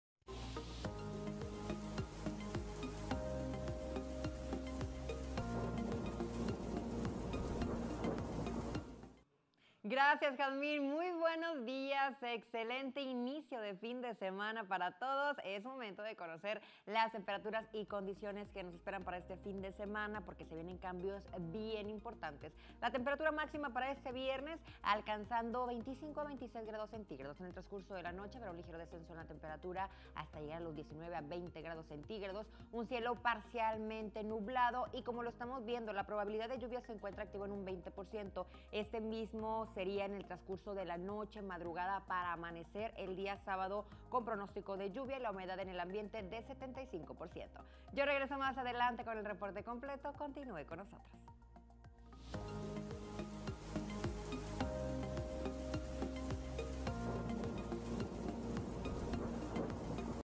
Avance clima 12 Noviembre Yulissa Castillo
Yulissa Castillo nos mantiene bien informados con lo mas relevante del clima de Monterrey y su área metropolitana.